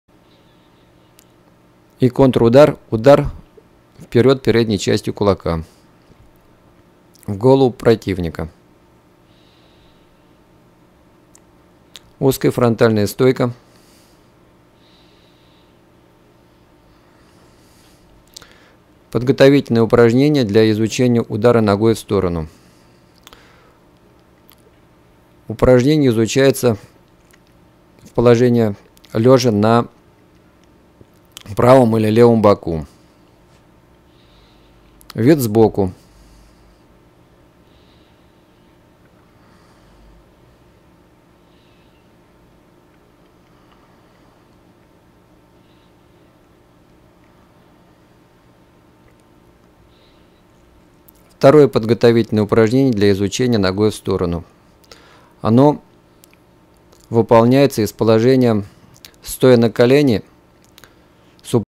training of punches and kicks the film 1 chunk 13
Disclosed program initial training course fighting. The material is divided on lessons, which allows to master consistently presented the program. In the film, attended by leading Russian grand master edinoborstv- Tadeusz Kasyanov, Vladimir Kovalev, Vladimir Kuznetsov, Sergei Ivanov-Katansky. The film can be used as part of high school obucheniya- in the form of extracurricular activities, and as a specialized discipline, as well as in different sections of martial arts and self-defense, in the power structures.